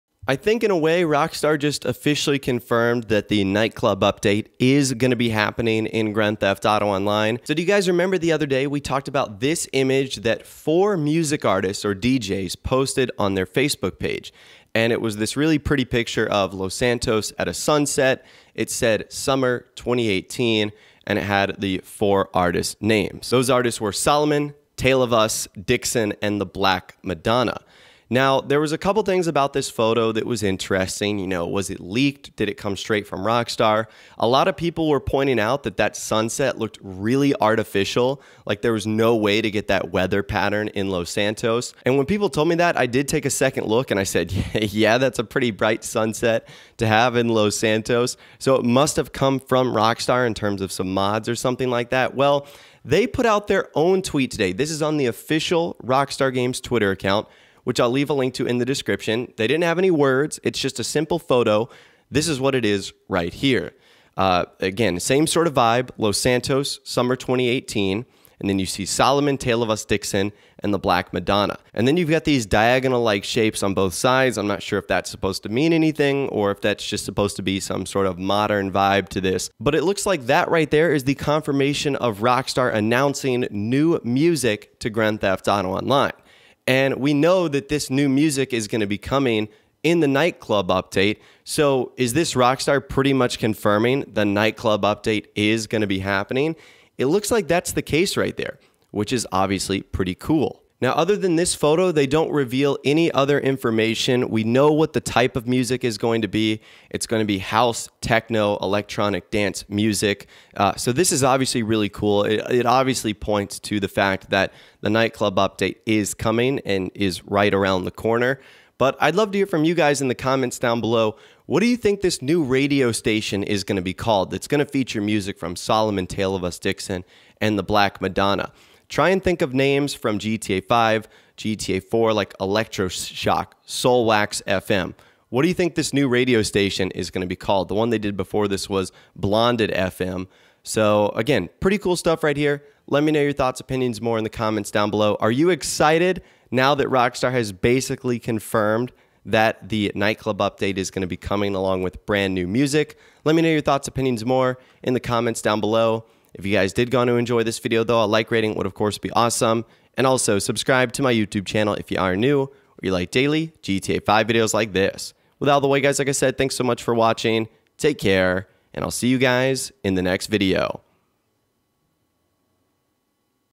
Rockstar Officially CONFIRMS Nightclub Update Coming To GTA Online? NEW Content Coming Summer 2018!
Rockstar Officially CONFIRMS Nightclub Update Coming To GTA Online? NEW Content & Music Coming This Summer 2018!

Rockstar just Tweeted out a picture that confirms NEW music will be coming to Los Santos This Summer 2018! Does this CONFIRM The Nightclub DLC as well?